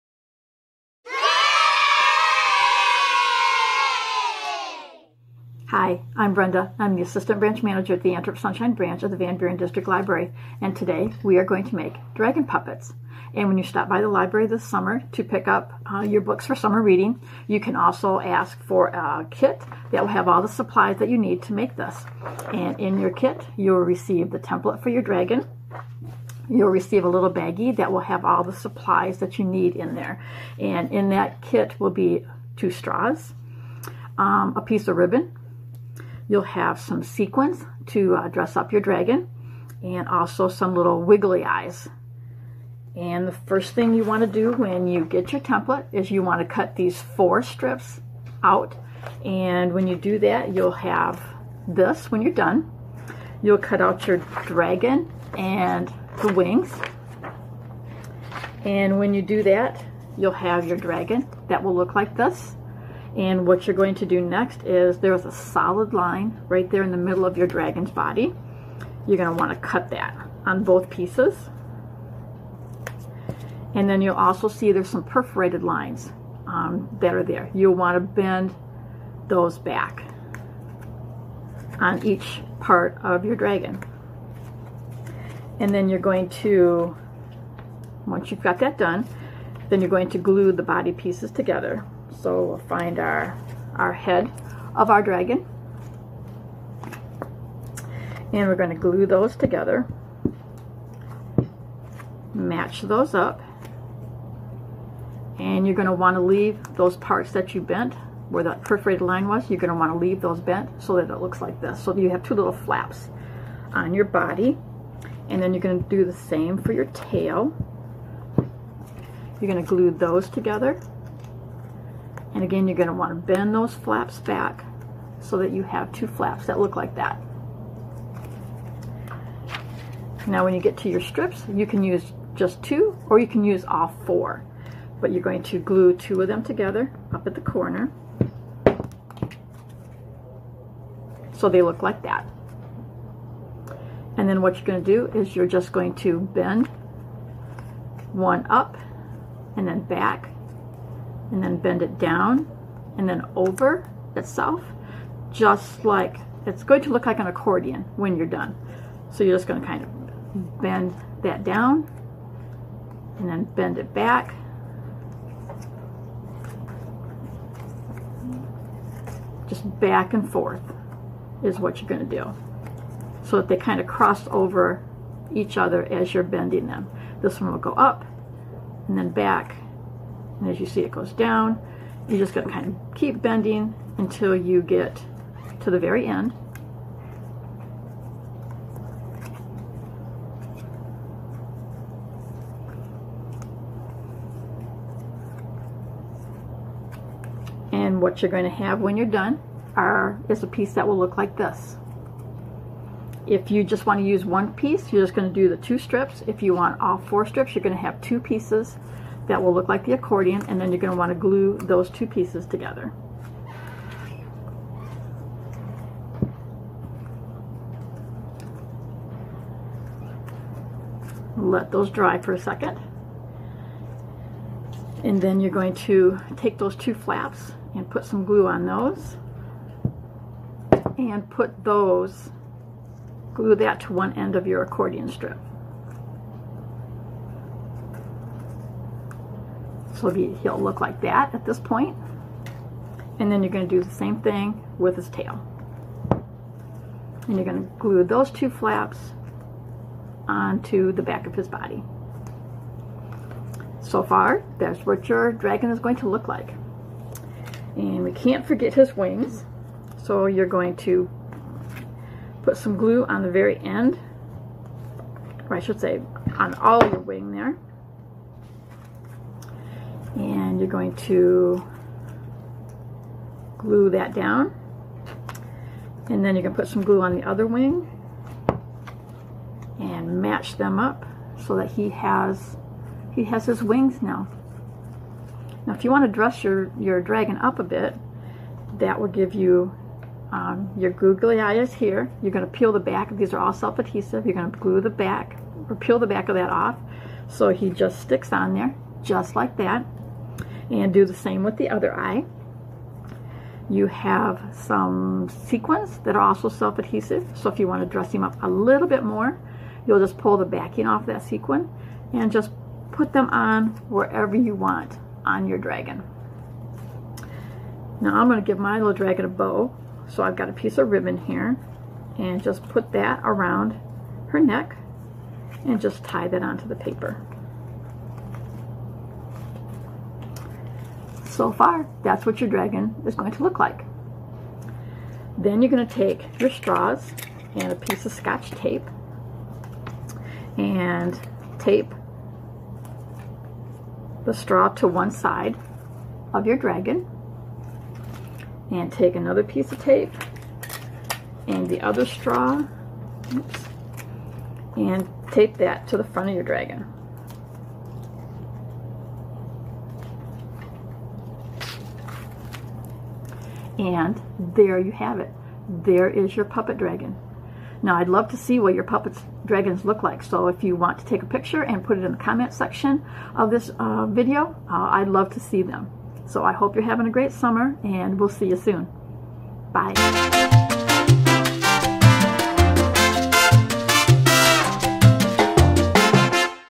VBDL's Online Crafts for Kids - July 16, 2020
Welcome to VBDL's Online Crafts for Kids! Miss Brenda instructs how to make a dragon puppet. Supplies needed: dragon puppet picture printed on card stock, straws, googly eyes, stickers/gems, ribbon, glue and coloring supplies!